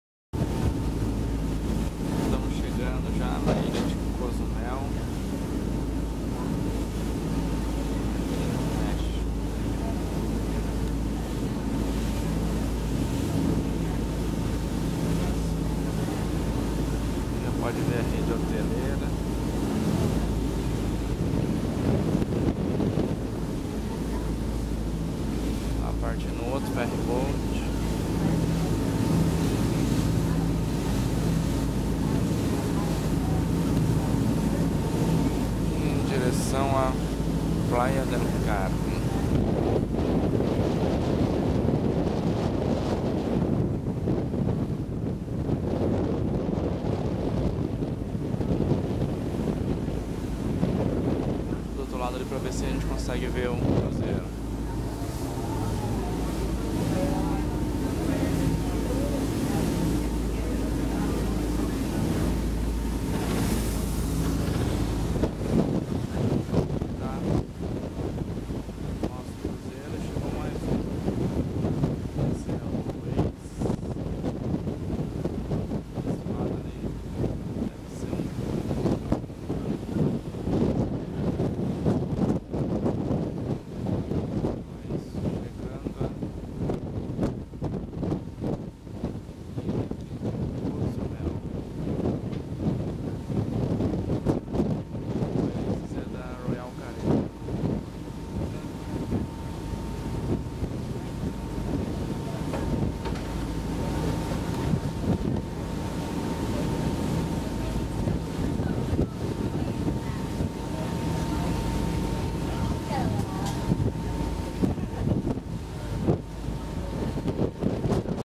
COZUMEL MÉXICO PASSEIO DE FERRY BOAT NO MAR DO CARIBE POR DUONE LATINO
Fica situada perto da costa oriental da península de Iucatã. Está coberta por um espesso bosque e possui abundante pesca. Sua principal atividade é o turismo, apresentando uma indústria hoteleira desenvolvida. Possui um aeroporto.
Localizada no Caribe mexicano, Cozumel é uma ilha plana composto de rocha vulcânica porosa. A elevação natural mais importante da ilha é inferior a 100 m. O cenotes formado pela infiltração de água através da rocha pedra há milhares de anos podem ser exploradas através de actividades como a natação ou mergulho, pode apreciar diversas espécies marinhas.
A ilha é de cerca de 48 km de norte a sul e 16 km de leste a oeste, tornando-se a terceira maior ilha do México após o Isla Tiburón, no estado de Sonora e Isla Angel de la Guarda na Baja California. Localizado a 20 km a leste da Península de Yucatán, e 60 km ao sul de Cancún, tem uma área de 647,33 km² e 1208 km² em comparação com o a Isla Tiburón e 895 km² da Isla Angel de la Guarda.